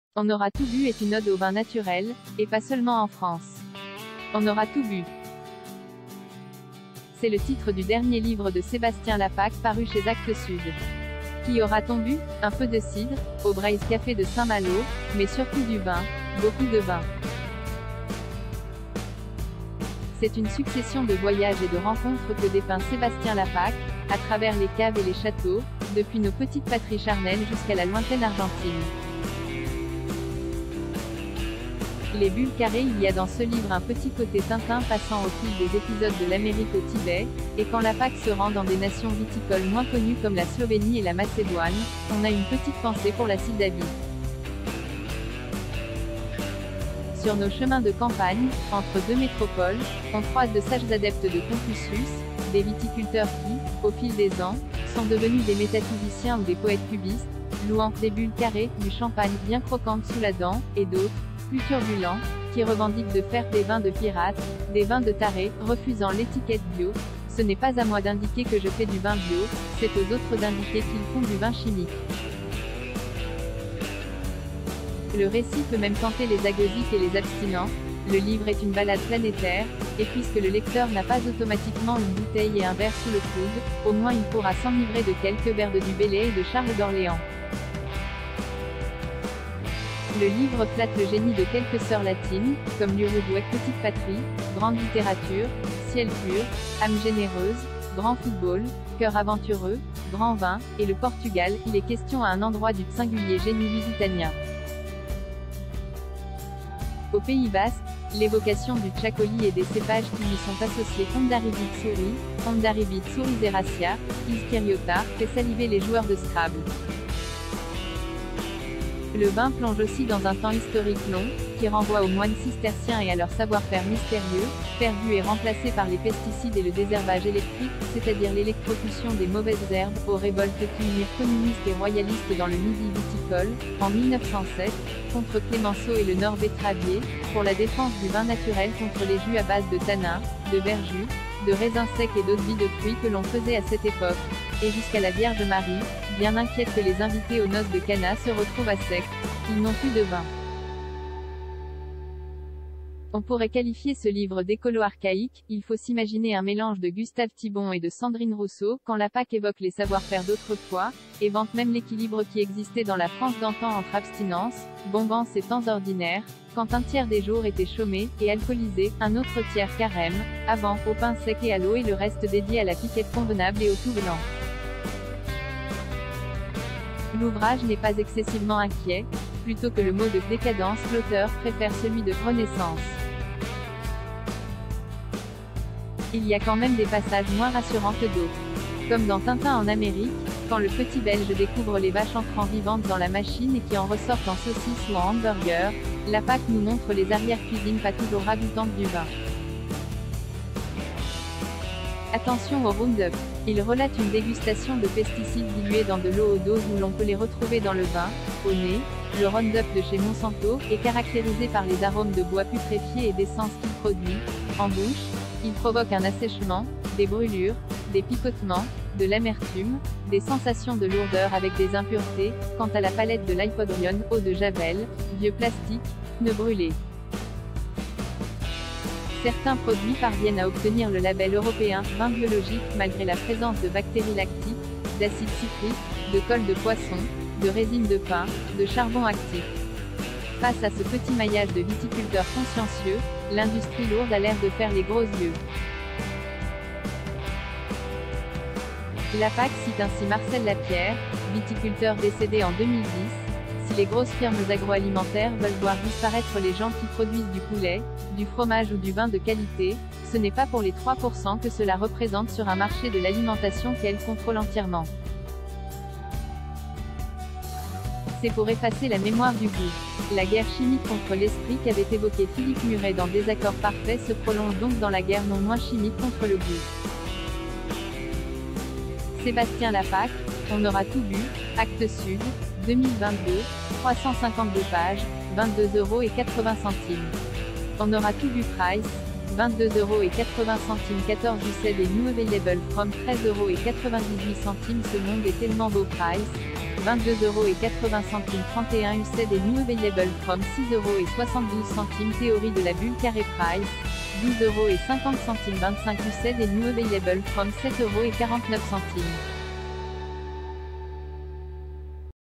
Sébastien Lapaque, la légende du saint buveur
Météo du Sébastien Lapaque, la légende du saint buveur
  On aura tout bu est une ode au vin naturel, et pas seulement en France.   On aura tout bu. C’est le titre du dernier livre de Sébastien Lapaque paru chez Actes Sud. Qu’y aura-t-on bu? Un peu de cidre, au Breizh Café de Saint-Malo, mais surtout du vin, beaucoup de vin. C’est une succession de voyages et de rencontres que dépeint Sébastien Lapaque, à travers les caves et les châteaux, depuis nos petites patries charnelles jusqu’à la lointaine Argentine.  Les bulles carrées   Il y a dans ce livre un petit côté Tintin passant au fil des épisodes de l’Amérique au Tibet, et quand Lapaque se rend dans des nations viticoles moins connues comme la Slovénie et la Macédoine, on a une petite pensée pour la Syldavie. Sur nos chemins de campagne, entre deux métropoles, on croise de sages adeptes de Confucius, des viticulteurs qui, au fil des ans, sont devenus des métaphysiciens ou des poètes cubistes, louant « des bulles carrées » du champagne « bien croquantes sous la dent » et d’autres, plus turbulents, qui revendiquent de faire « des vins de pirates, des vins de tarés », refusant l’étiquette bio : « ce n’est pas à moi d’indiquer que je fais du vin bio, c’est aux autres d’indiquer qu’ils font du vin chimique ».   Le récit peut même tenter les agueusiques et les abstinents : le livre est une ballade planétaire, et puisque le lecteur n’a pas automatiquement une bouteille et un verre sous le coude, au moins il pourra s’enivrer de quelques vers de Du Bellay et de Charles d’Orléans. Le livre flatte le génie de quelques sœurs latines, comme l’Uruguay (« petite patrie, grande littérature, ciels purs, âmes généreuses, grand football, cœurs aventureux, grands vins ») et le Portugal (il est question à un endroit du « singulier génie lusitanien »).   Au Pays basque, l’évocation du txakoli et des cépages qui lui sont associés (hondarribi zuri, hondarribi zuri zerratia, izkiriota) fait saliver les joueurs de Scrabble. Le vin plonge aussi dans un temps historique long, qui renvoie aux moines cisterciens et à leur savoir-faire mystérieux, perdus et remplacés par les pesticides et le désherbage électrique (c’est-à-dire l’électrocution des mauvaises herbes) ; aux révoltes qui unirent communistes et royalistes dans le Midi viticole, en 1907, contre Clemenceau et le Nord betteravier, pour la défense du vin naturel contre les jus à base de tannins, de verjus, de raisins secs et d’eau-de-vie de fruits que l’on faisa